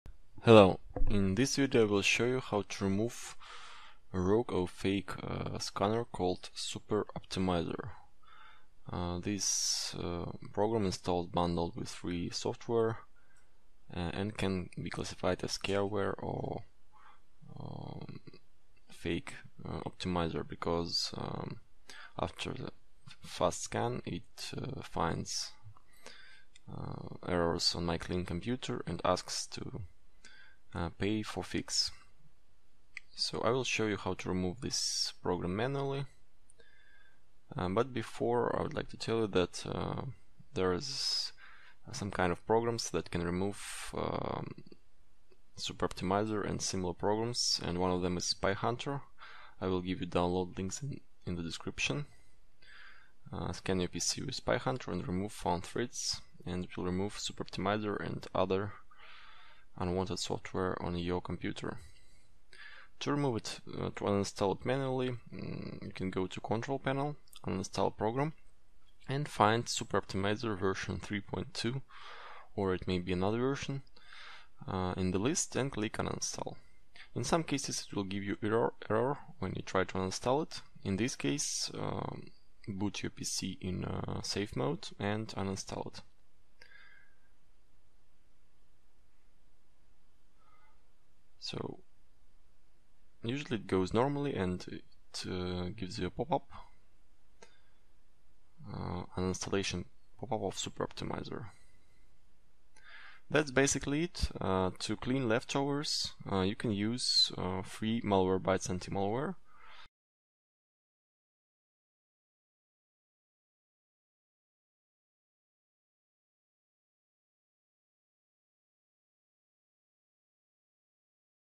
How to uninstall (remove) Super Optimizer (Windows 7, 8, 10, XP)
Complete and actual tutorial that will show how to remove Super Optimizer and all leftover items (files and folders) in popular browsers Internet Explorer, Firefox, Google Chrome.